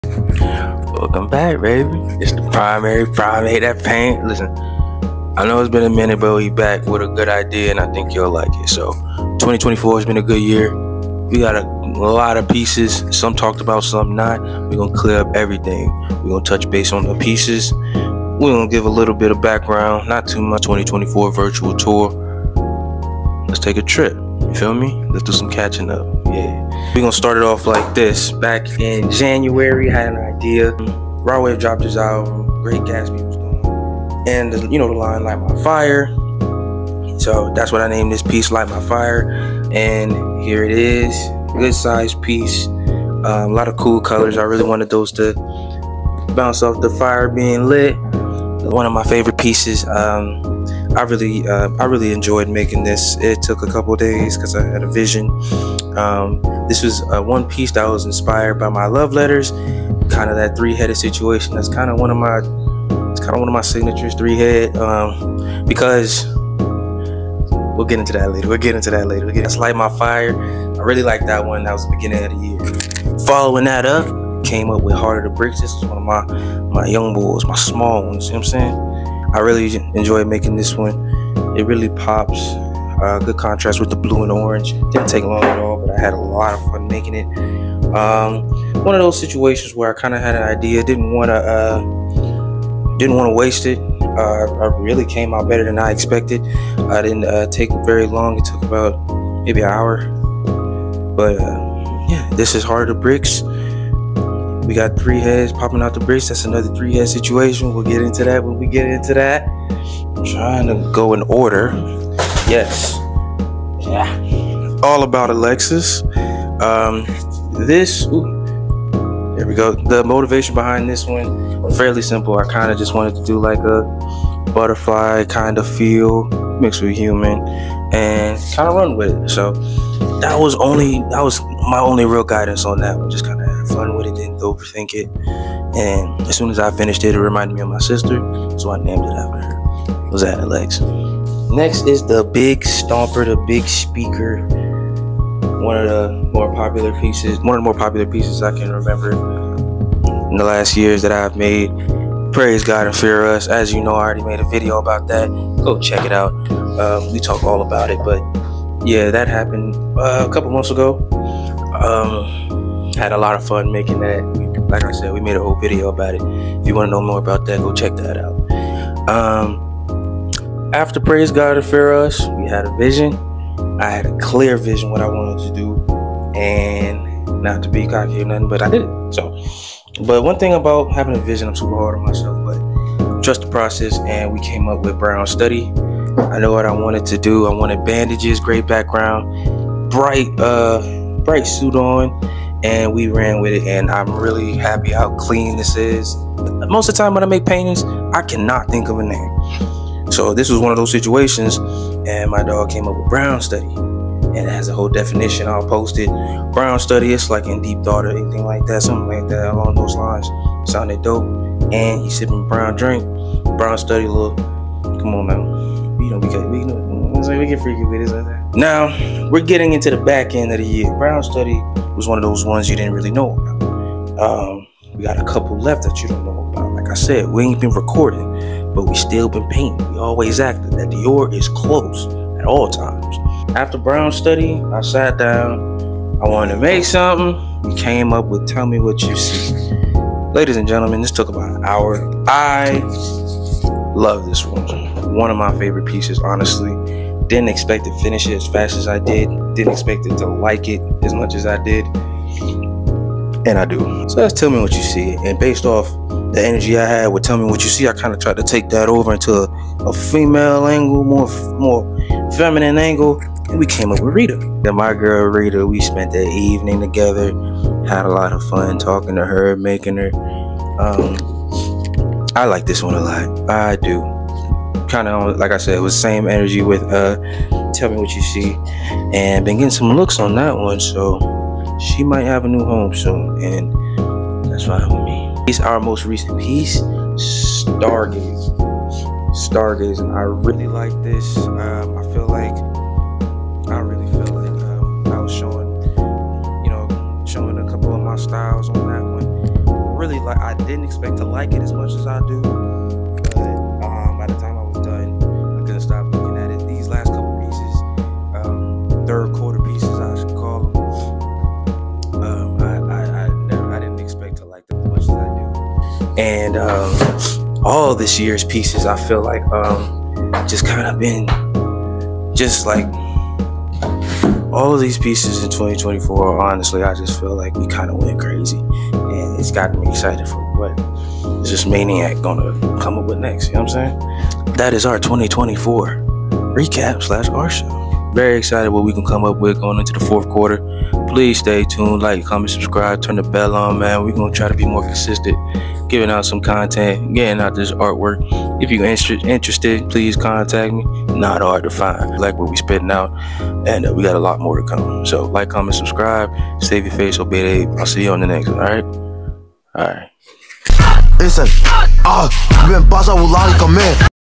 2024 Art Show/Recap
come catch up with the primary primate!!
I know it’s been a while since my last video but I’m back with more art. Quick little catch up on what’s been going on.

What year should the next art show be?